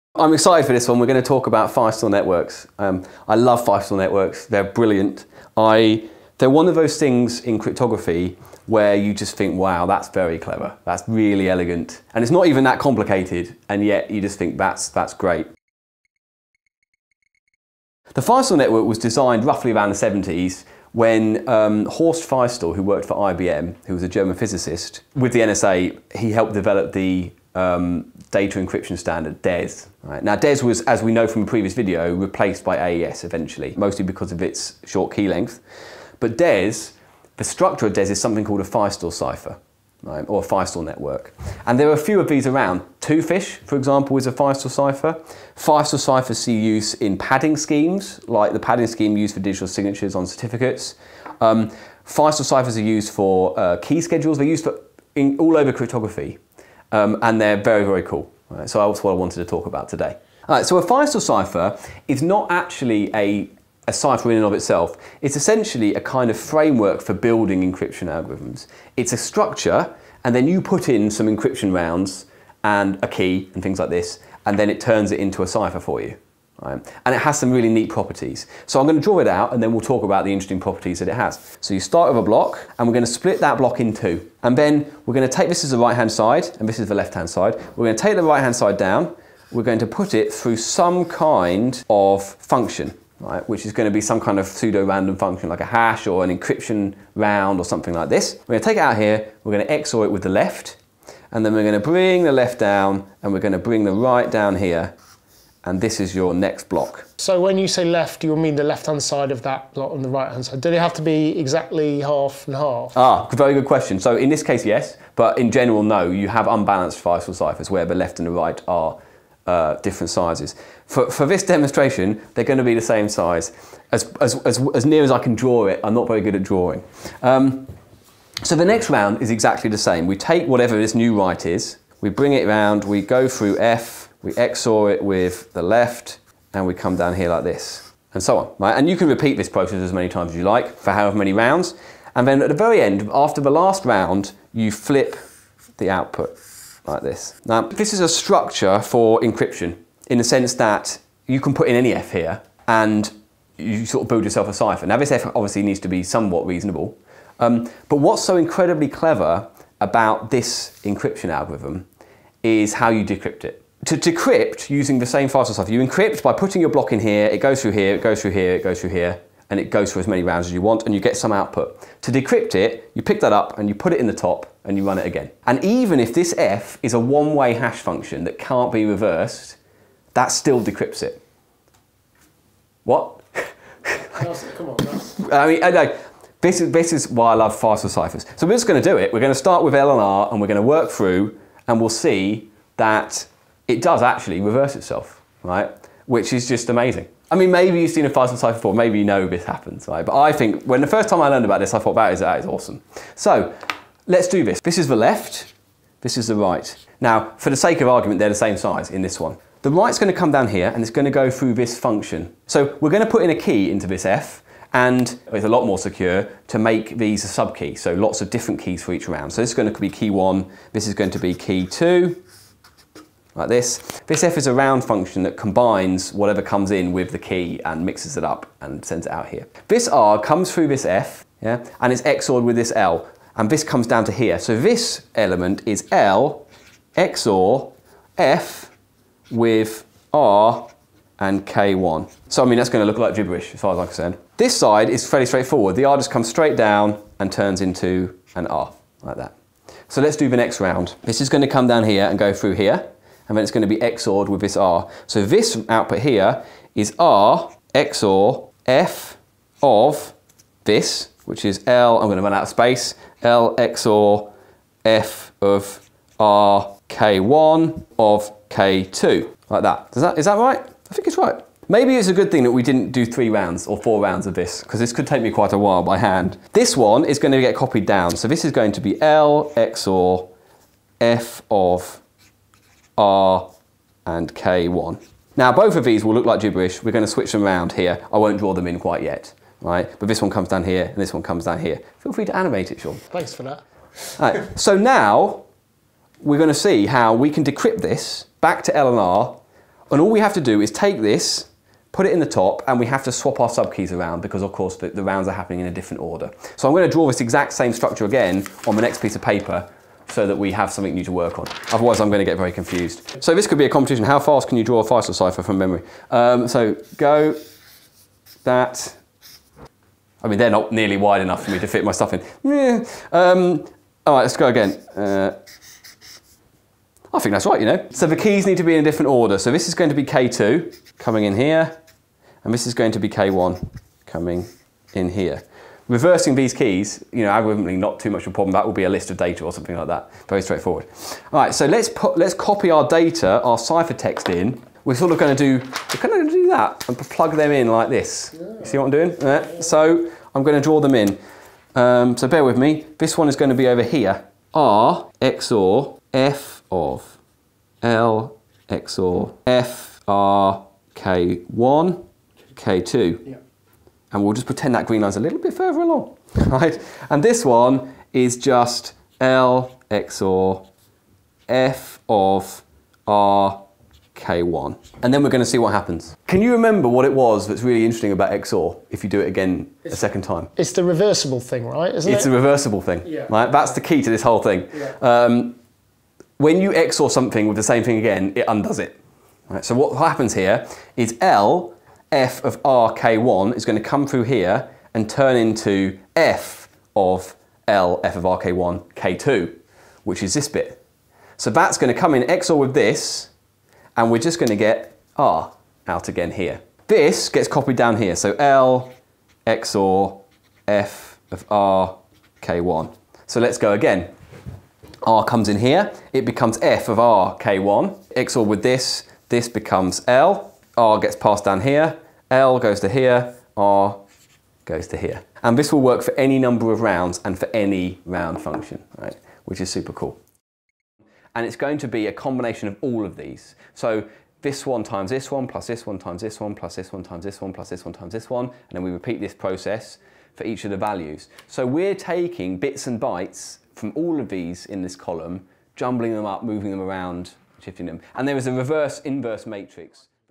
Feistel Cipher - Computerphile
One of the most elegant solutions for cryptography. Dr Mike Pound explains one of his most favourite ciphers.